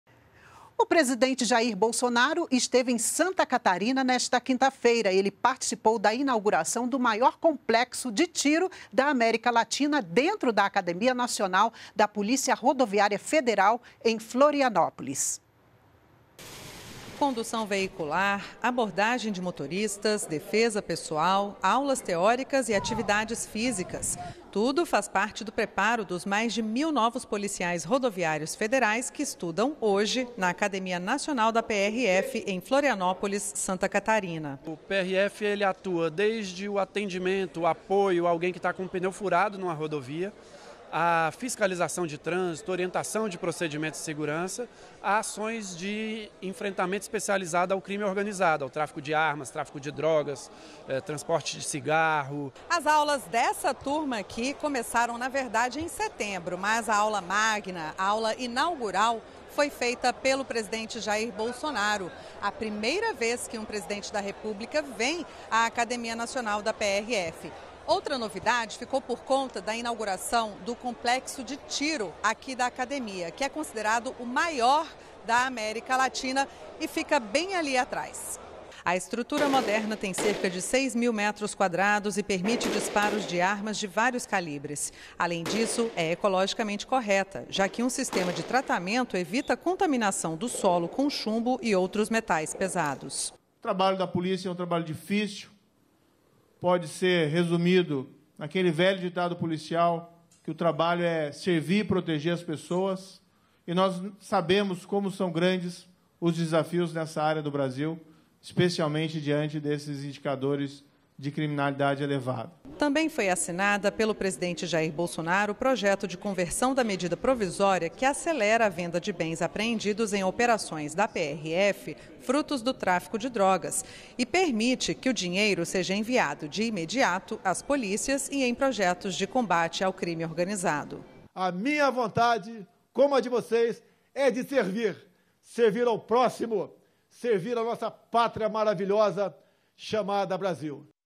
Presidente Jair Bolsonaro participa de inauguração de Complexo de Tiro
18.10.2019 -  Presidente Jair Bolsonaro esteve em Santa Catarina nesta quinta-feira, onde participou da inauguração do maior Complexo de Tiro da América Latina, dentro da Academia Nacional da Polícia Rodoviária Federal, em Florianópolis.